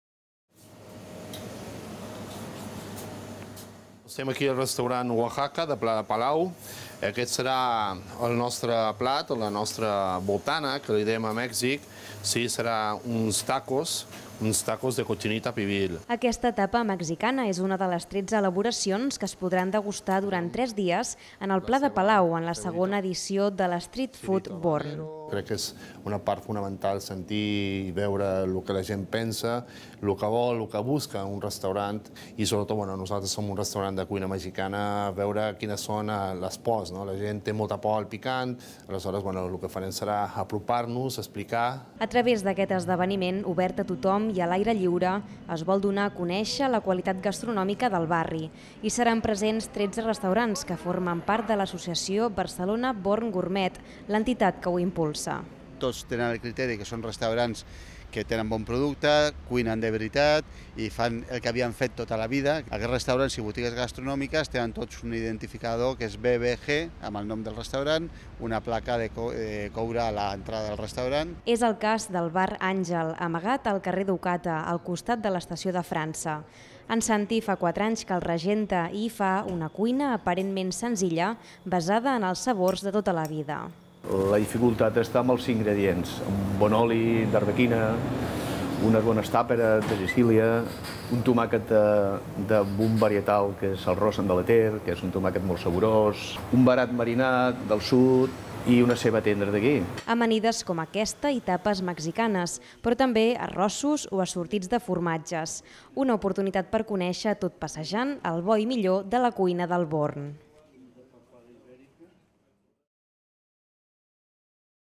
Degusta el bo i millor a l'Street Food Born
Del 19 al 21 de juny a Pla de Palau es poden degustar tapes elaborades a 13 restaurants del Born. Es tracta de la 2a edició de l’Street Foot Born, una iniciativa impulsada per Barcelona Born Gourmet per donar a conèixer la qualitat gastronòmica del barri. Més informació